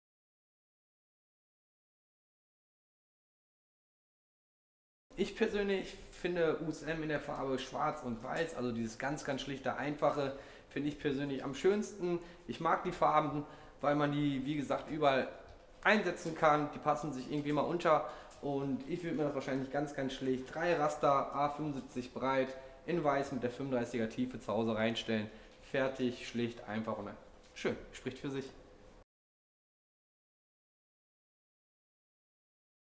Schlichtheit des USM „Haller" // Drifte Experten Team
Drifte Teammitglied Christof erzählt Ihnen in diesem Video, wie er den „Haller" konfektionieren würde. Mehr zum USM Beistellmöbel gibt es

Der Regal von USM ist bei Drifte in Moers erhältlich:

Drifte Wohnform GmbH
Holderberger Straße 88
47447 Moers-Kapellen